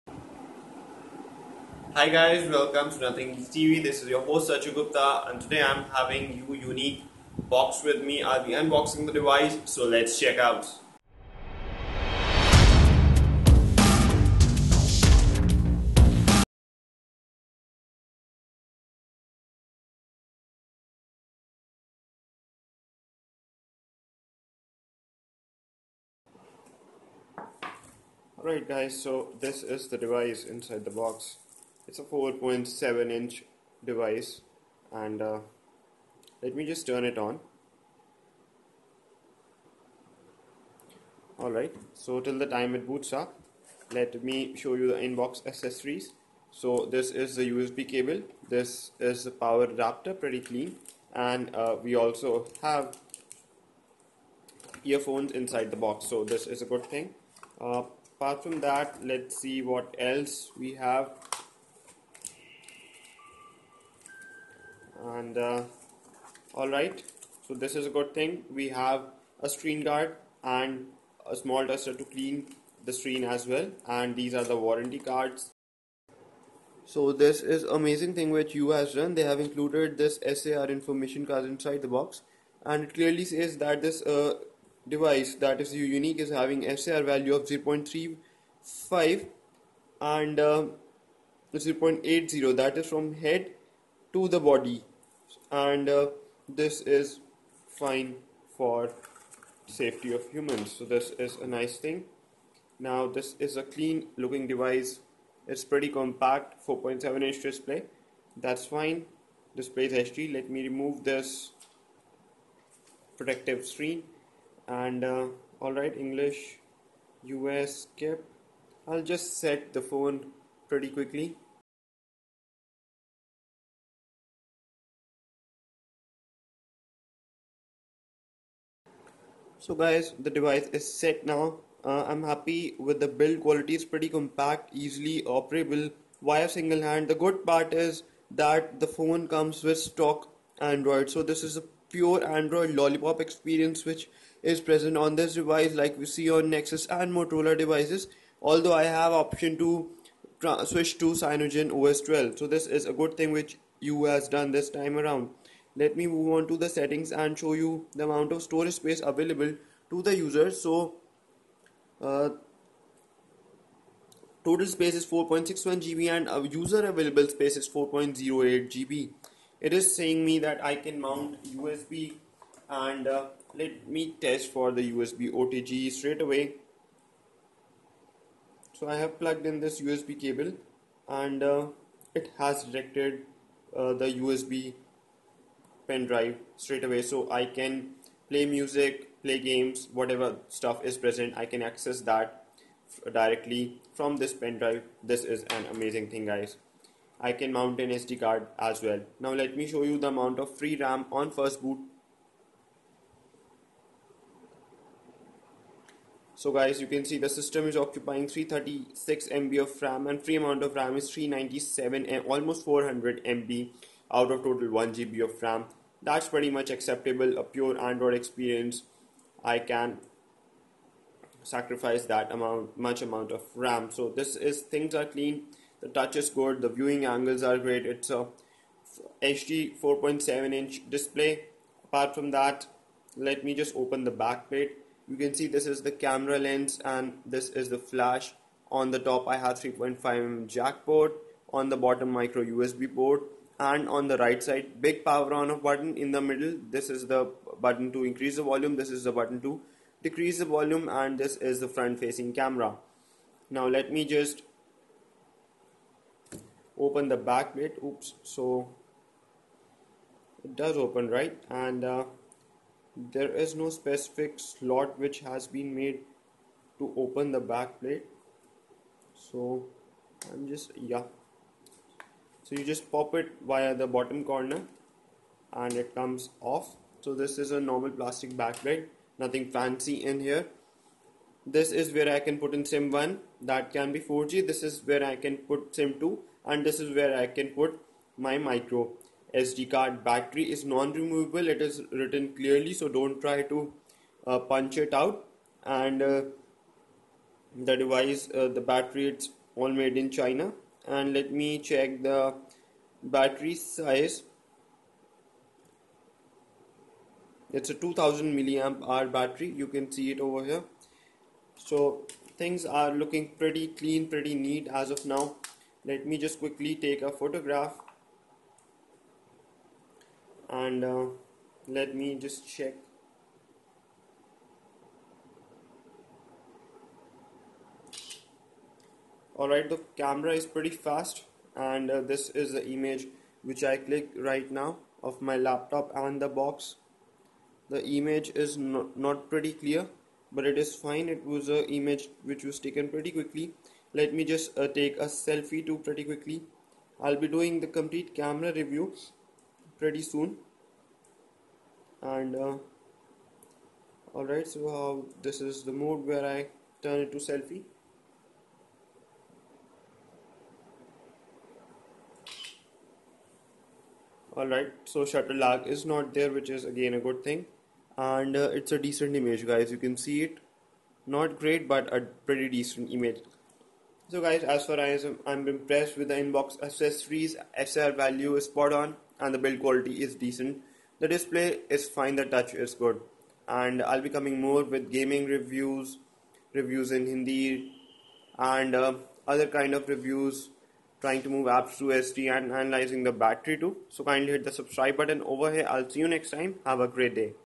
Yu Yunique Unboxing and hands on review
This phone is a low cost 4G device in India. The USP is that the phone got a 4.7 inch HD display and I am pretty happy with the colours and the touch.

The Yu Yunique is a compact device and gives the users a pure Android experience in the form of stock Android Lollipop.However, you can also switch to Cyanogen OS on this phone.